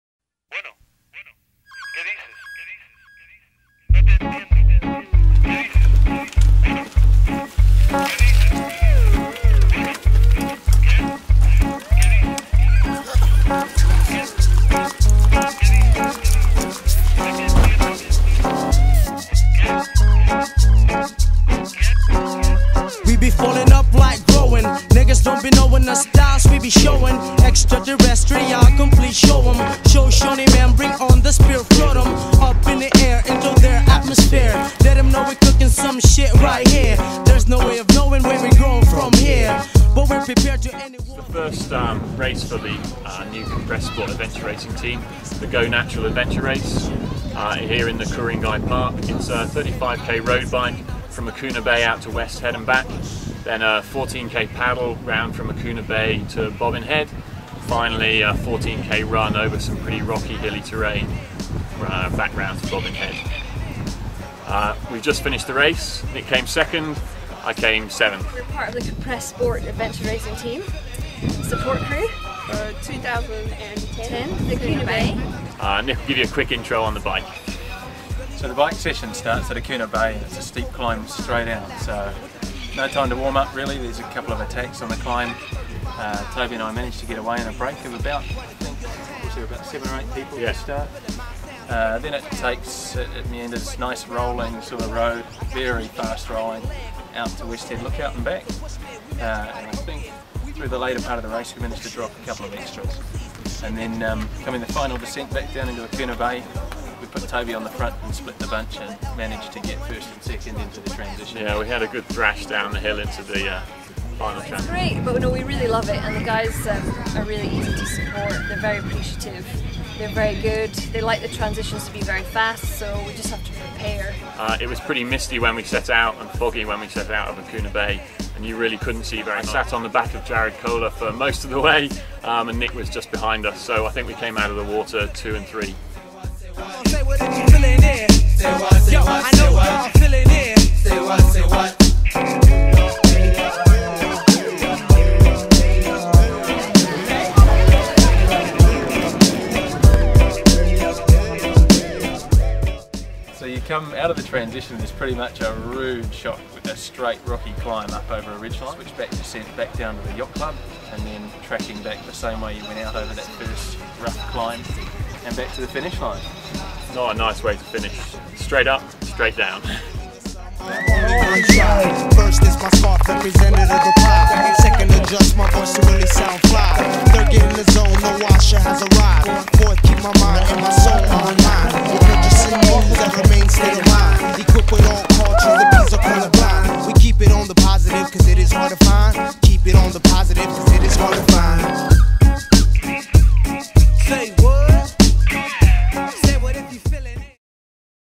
Go Natural Multisport 2010
The opening event for the newly formed Compressport Adventure Racing Team. Toby and Nic talk about the race and how it unfolded.